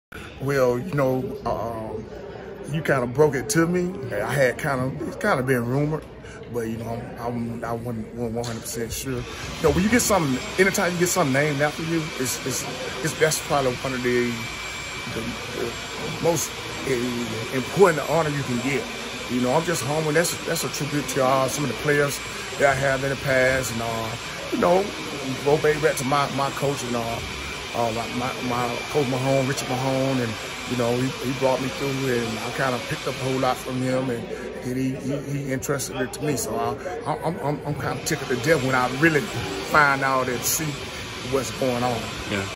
George Washington Carver High School in Columbus, GA names basketball court after retiring coach
Carver recently named its basketball court after retiring boys basketball coach Warren Beaulah. Here's Beaulah's reaction to the news. More from the Ledger-Enquirer: